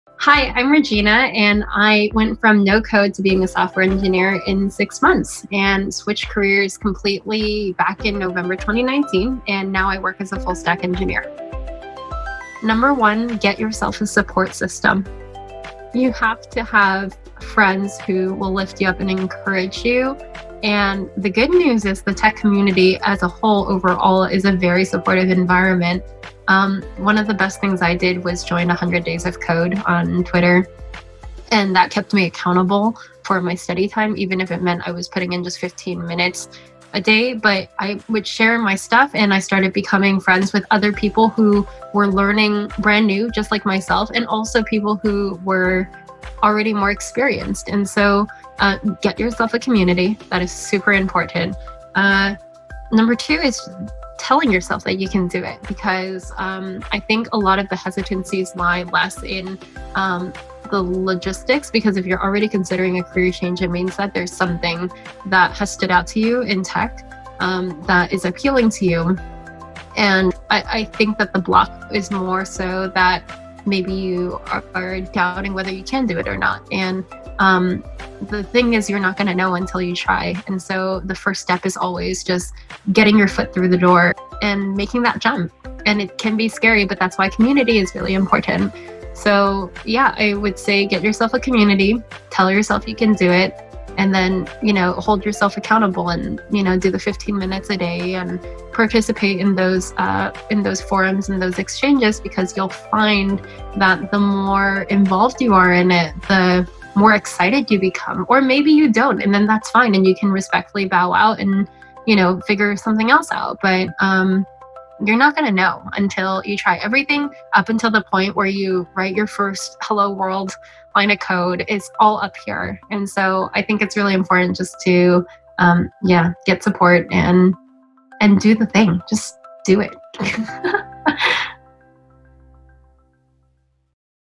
Career change advice from a pianist turned software engineer
Codecademy community member Regina Gerbeaux recently went from being a classical piano teacher with no coding experience to landing a job as a software engineer in six months. We sat down to talk with Regina over Zoom about her inspiring journey.